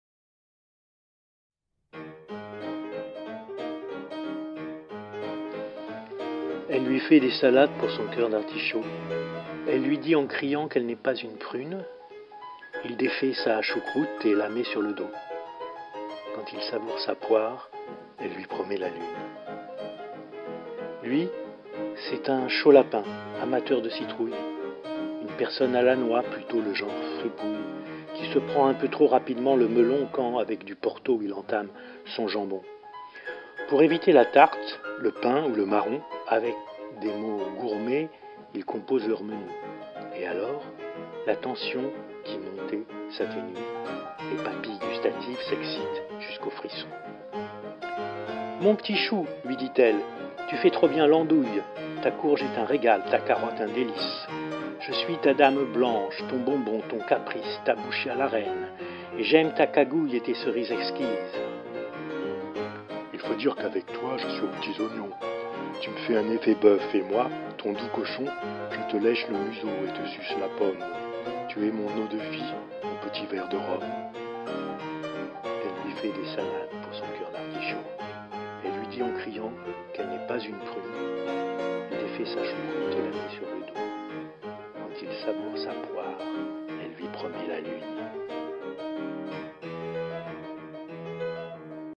quand il savoure sa poire poème de Gérard Netter
Poème de et lu par Gérard Netter - recueil "Fragments"